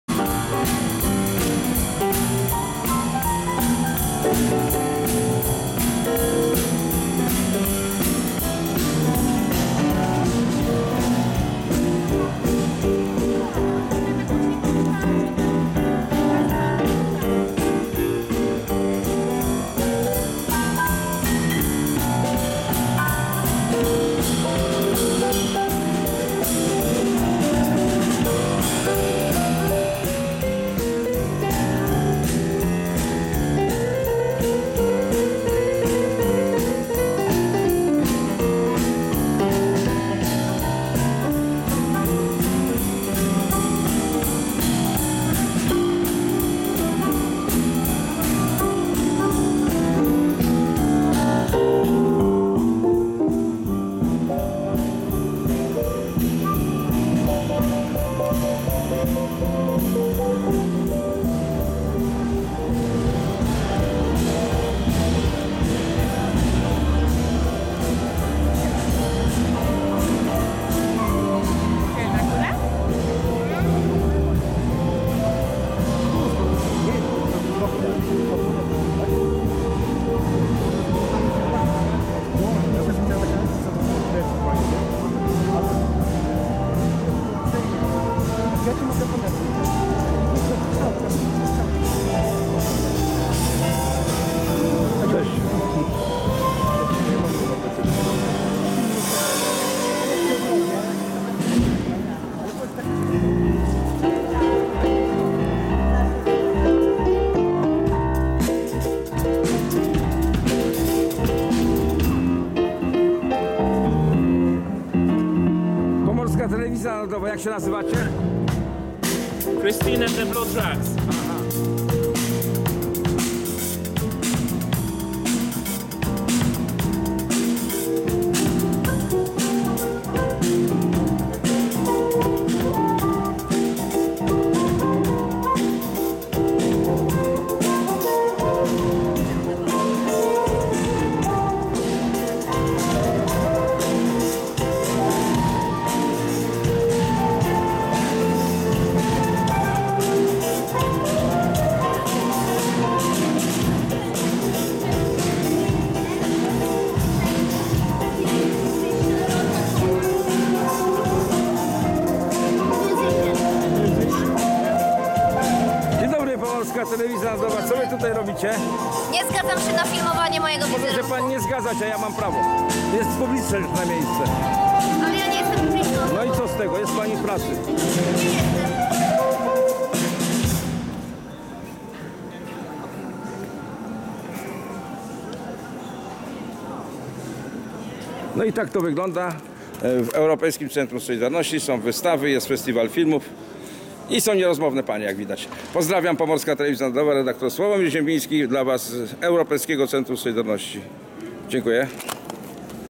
EUROPEJSKIE CENTRUM SOLIDARNOŚCI - FESTIWAL FILMÓW FABULARNYCH I MUZYKA
W dniu 19-go października 2019 r. poproszono mnie abym oprowadził grupę osób przybyłych z różnych miast w Polsce po terenie Stoczni Gdańskiej.Odwiedziliśmy razem historyczna Sale BHP , gdzie w sierpniu 1980 r. podpisano Porozumienie Gdańskie.Ponieważ byłem na tej sali wówczas delegatem OBR Meprozet w Gdańsku , jako przywódca strajku w tej firmie i członkiem MKS-u a blisko współpracowałem z Joanną i Andrzejem Gwiazdą mogłem tym ludziom opowiedzieć , jak było wówczas naprawdę w sierpniu 1880 r. Później przeszliśmy do Europejskiego Centrum Solidarności , gdzie odbywał się festiwal filmów fabularnych i właśnie emitowano film .kryminalny!?Obejrzeliśmy tez wystawę stałą o strajku 1980 w Stoczni Gdańskiej wówczas imienia Włodzimierza Lenina.Zapytałem uczestników spotkania ze mną o ich wrażenia po przejściu przez wszystkie ekspozycje.

Pomorska TV NARODOWA
red.Sławomir Ziembiński